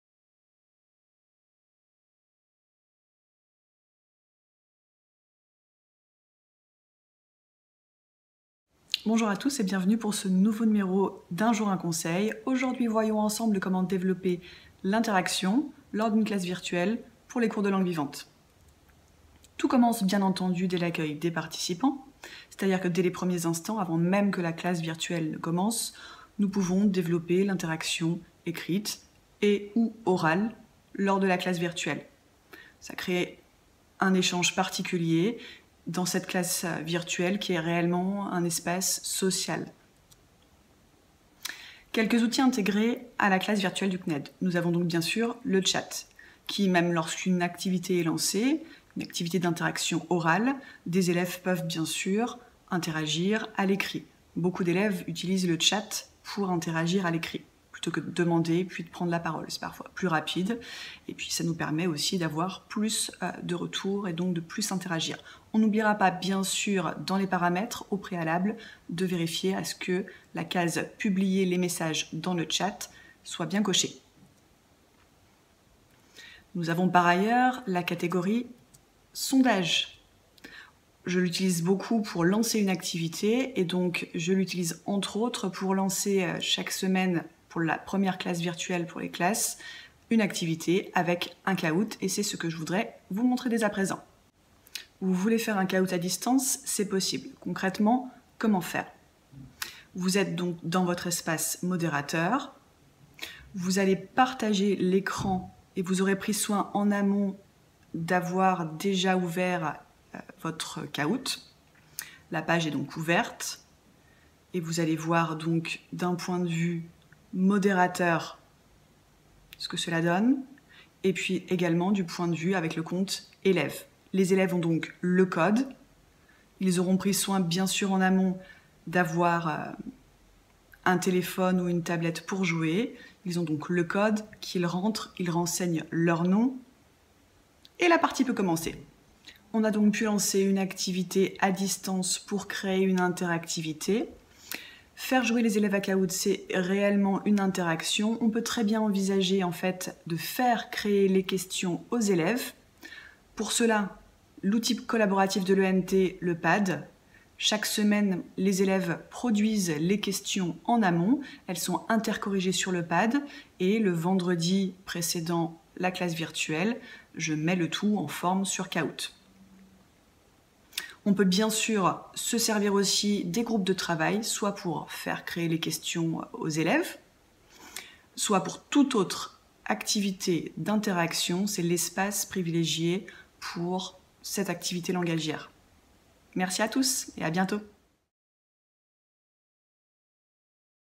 Cours de langues en classe virtuelle
Aujourd'hui Amélie Beney, professeure, nous conseille sur l'organisation d'un cours de langues en classe virtuelle - Dane de Versailles - avril 2020
"Kahoot est une application qui nécessite la création d’un compte auprès d’un service tiers. La création de compte(s) implique une collecte ou un traitement de données à caractère personnel (par le service tiers) pour lesquels le consentement des utilisateurs est nécessaire."